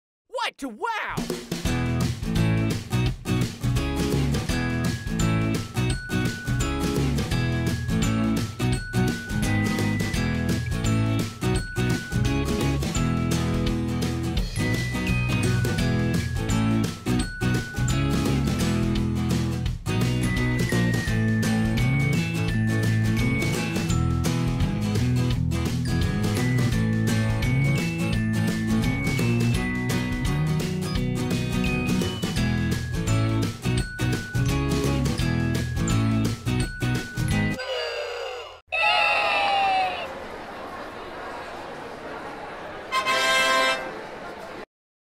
Rainbow Tie-Dye Shoes - Create with Tulip
Step on the scene in style with rainbow tie-dye shoes! See how easy it is to get the look with Tulip One-Step Tie Dye and a paintbrush.